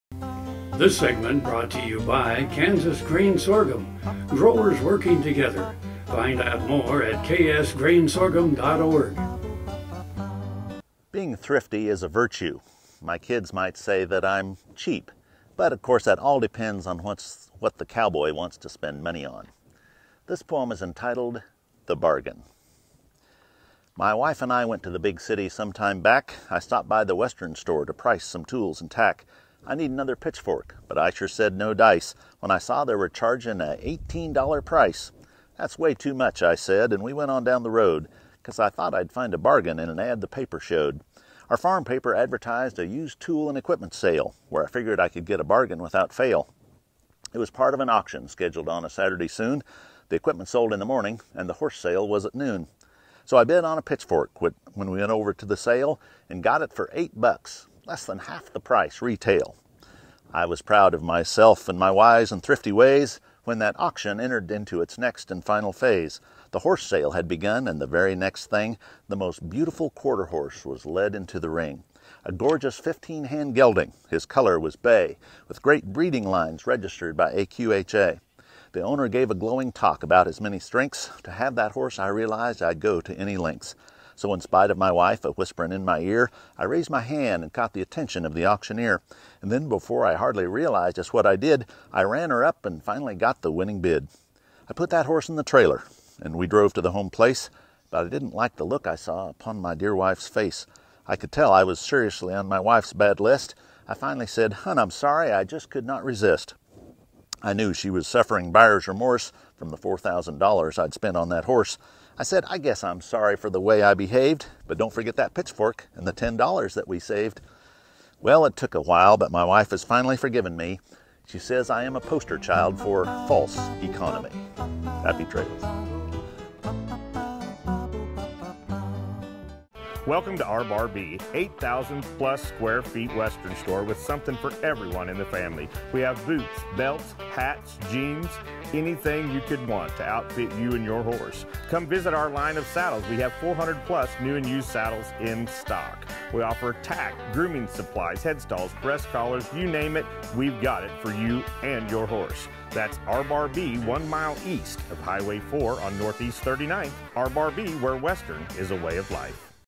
Around Kansas - Ron Wilson Bargin Poem - 3/14/18
Ron: Being thrifty is a virtue. My kids might say that I'm cheap. Of course, that all depends on what the cowboy wants to spend money on. This poem is entitled The Bargain. My wife and I went to the big city sometime back. I stopped by the western store to price some  tools and tack. I need another pitchfork, but I sure said no dice. When I saw they were charging $18 price, "That's way too much," I said. We went on down the road because I thought I'd find a bargain in an ad the paper showed.